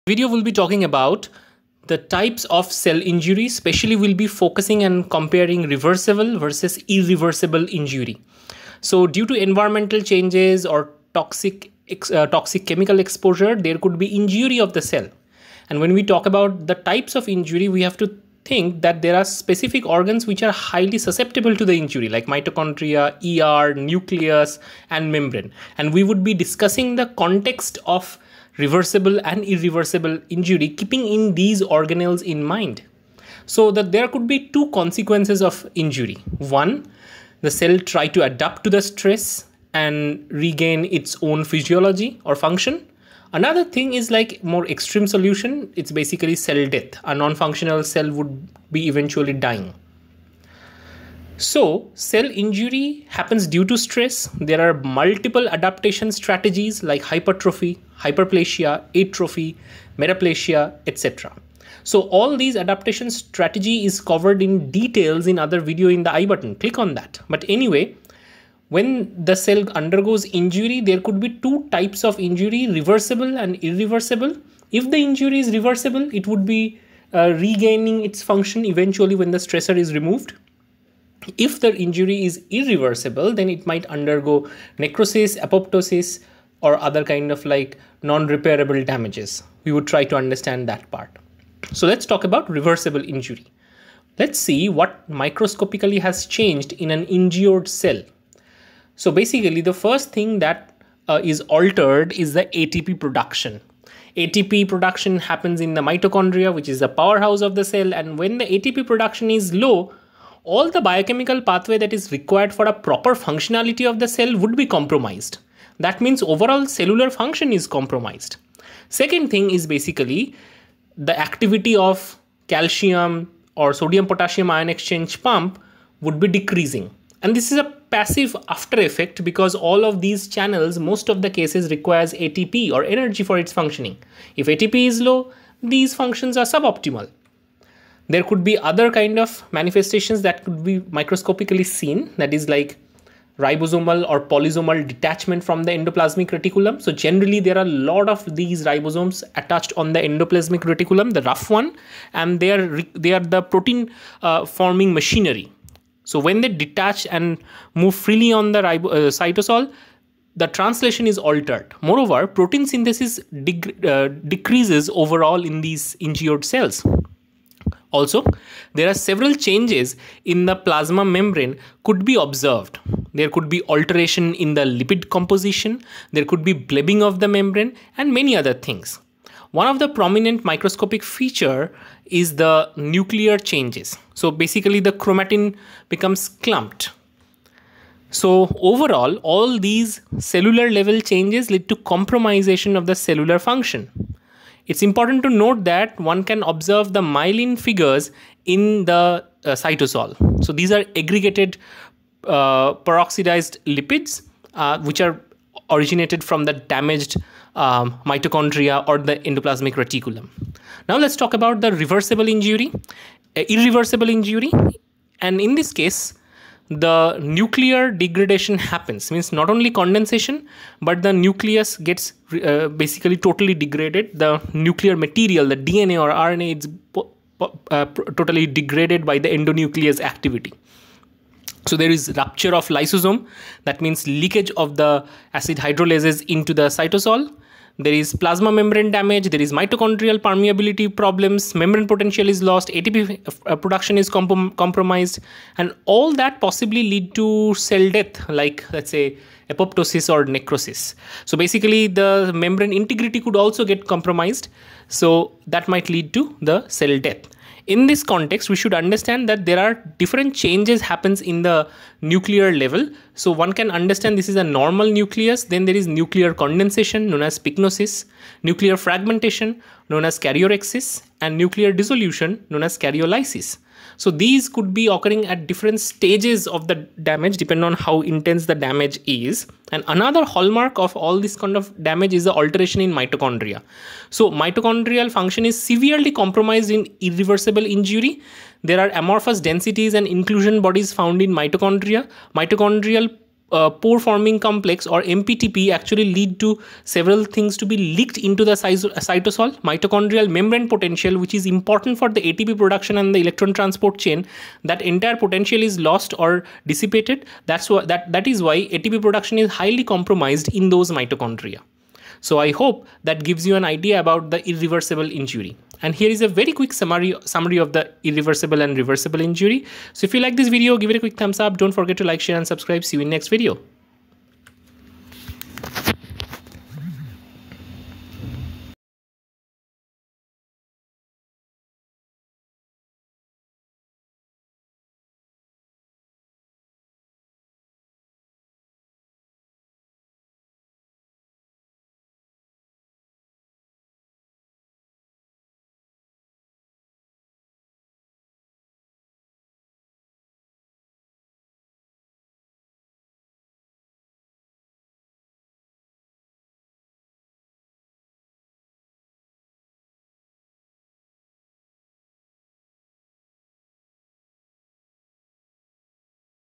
Reversible vs irreversible cell injury | Pathology | USMLE
This video talks about the Reversible vs irreversible cell injury